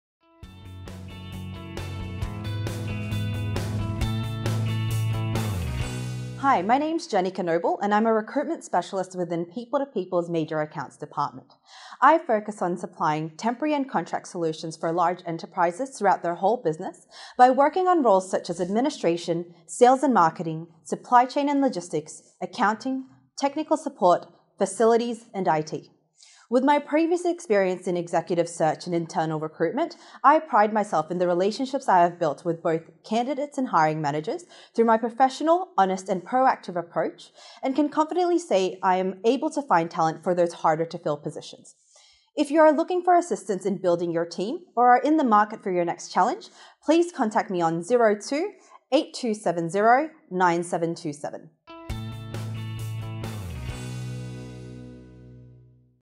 Jennika Noble - people2people
Hi, My name is Jennika and I'm an account manager within people2people's major accounts department. I focus on supplying temporary and contract solutions for large enterprises throughout their whole business by working on roles such as; administration, sales and marketing, supply chain and logistics, accounting, technical support, facilities and IT.

With my previous experience in executive search and internal recruitment, I pride myself in the relationships I have built with both candidates and hiring managers through my professional, honest and proactive approach and can confidently say I am able to find talent for those harder to fill positions.